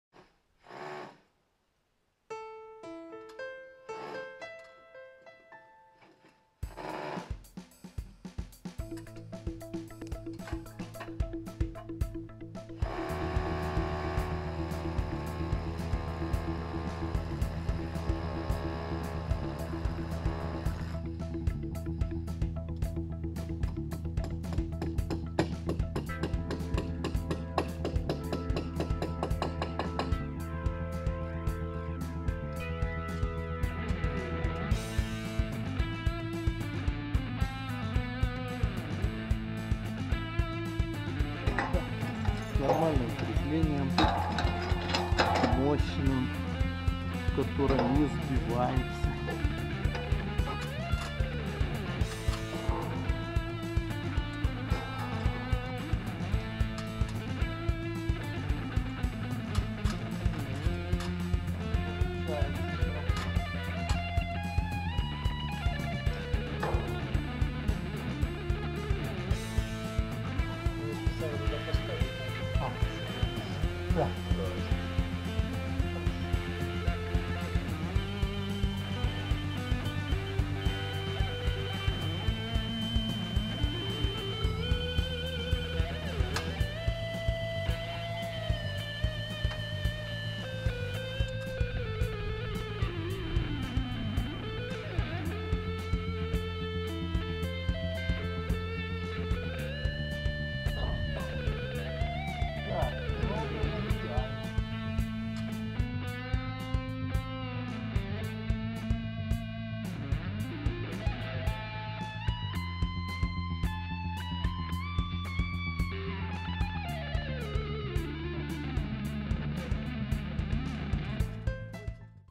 Установка "правильной" спутниковой антенны
Если хотите один раз поставить и забыть - ставьте только такие антенны!